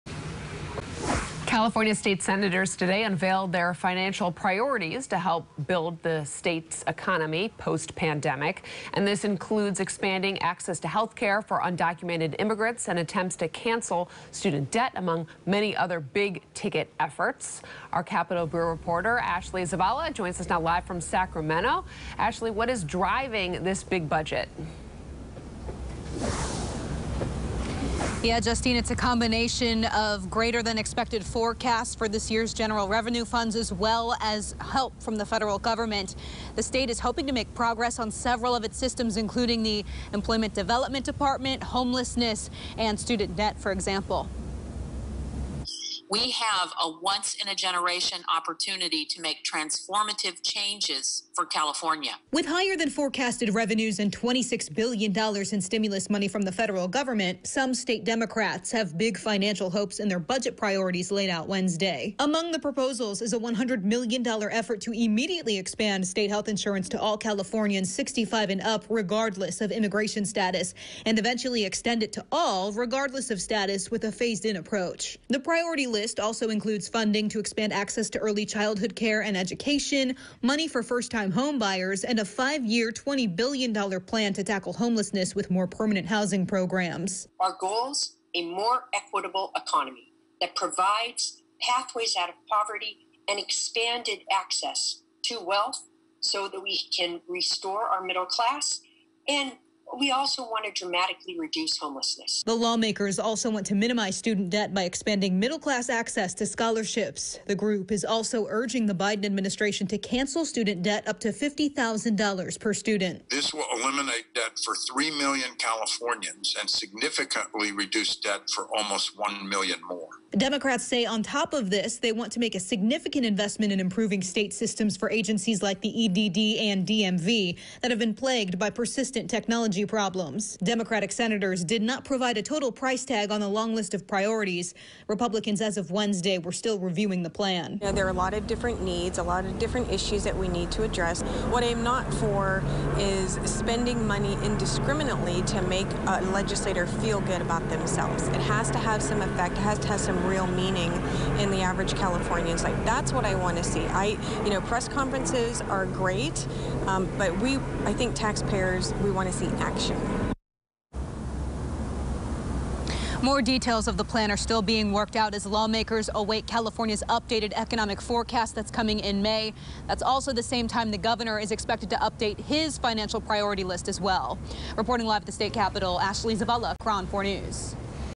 Rebuilding California's economy post-pandemic
California State Senators Wednesday unveiled their financial priorities to help build the state’s post-pandemic economy.